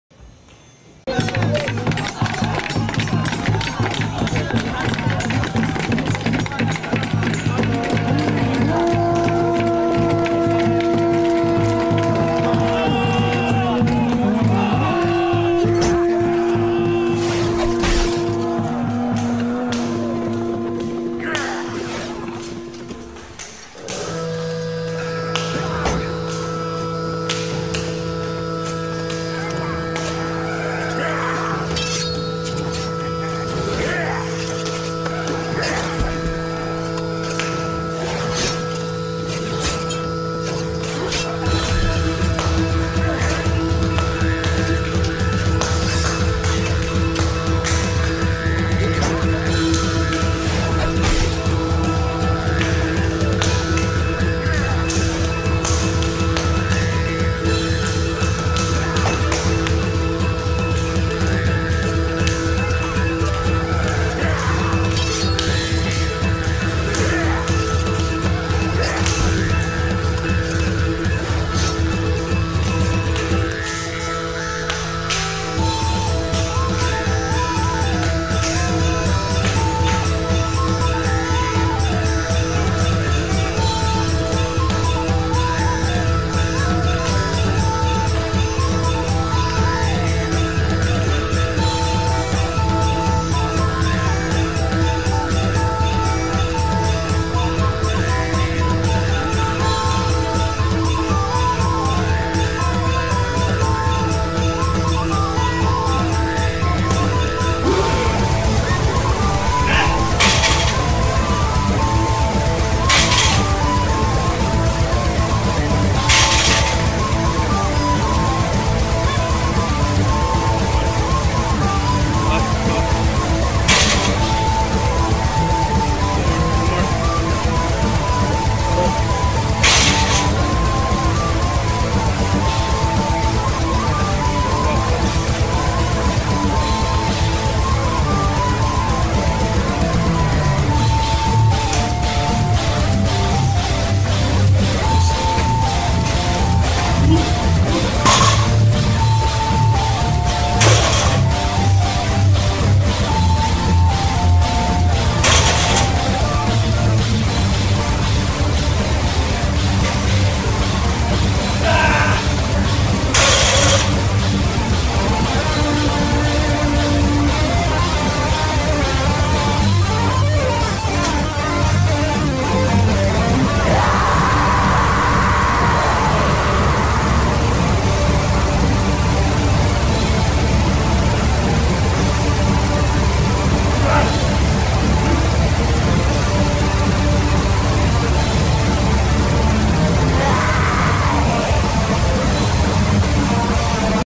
Russian Folk Dance / Training
-practicing/reviewing basic Russian Folk Dance drills/moves
-expect more of these drills on lower body/leg days
-folk dancing can be demanding on cardio, takes coordination and can tax the legs...great supplementary way to work out your quads/glutes/calves. also lots of fun!

SERVICES: Online Coaching & Custom Programs
ANY amount helps me live and make videos